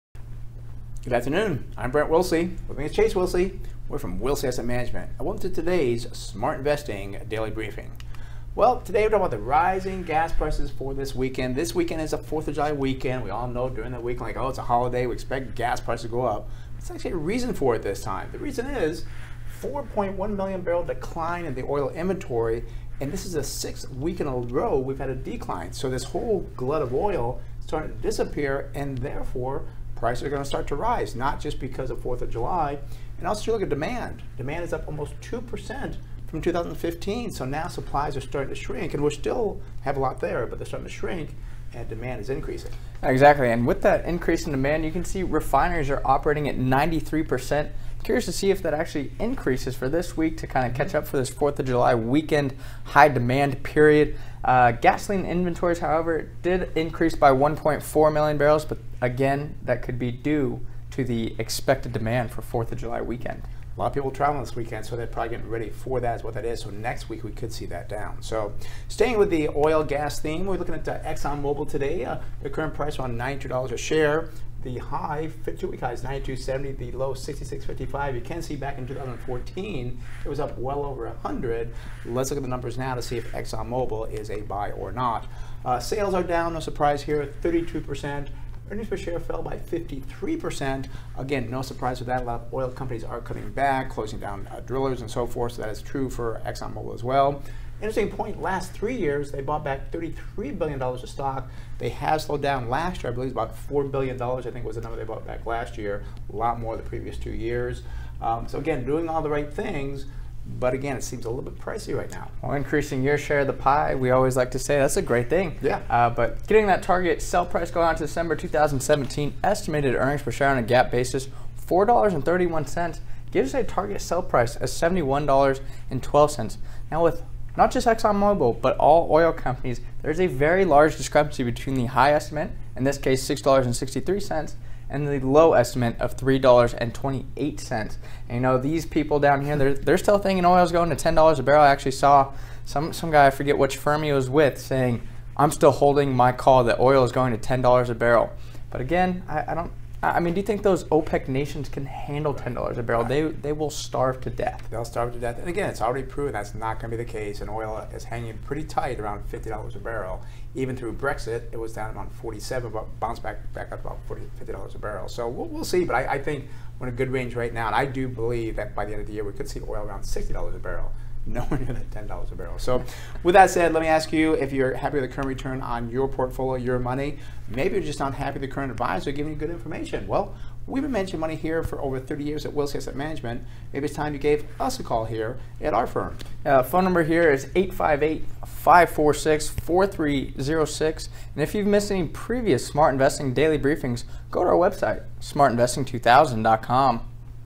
Why Oil Pops for 4th of July Weekend
Oil saw a big increase in price today, thanks to a surprise drawdown of 4.1 million barrels. This marks the sixth week in a row of inventory drawdowns. Demand is up 1.8% when compared to 2015. Refineries have been operating at 93% in order to keep up with demand for peak driving season. We saw an increase in gasoline inventories of 1.4 million barrels, which could be due to preparation for the increase in driving 4th of July weekend.

The company of the day is Exxon Mobil (XOM), whose current price is $92.46 and 52-week range is $66.55-$92.70. Sales have fallen 32.2% over the last twelve months, while EPS has fallen by 53.3%. Much of this can be attributed to the drop in oil prices and the cuts the company has made in drilling and exploration. The company has been very big on stock buy backs, and over the last 3 years they have repurchased over $33 billion worth of their stock. As we always say, this is great because it increases your piece of the pie and makes your stake in the company larger. Looking forward to December 2017, estimated EPS of $4.31 gives us a target sell price of $71.12. While this is below the current price, it is important to note the wide range between the high and low analyst estimates. The high estimate is $6.63, and the low estimate is $3.28.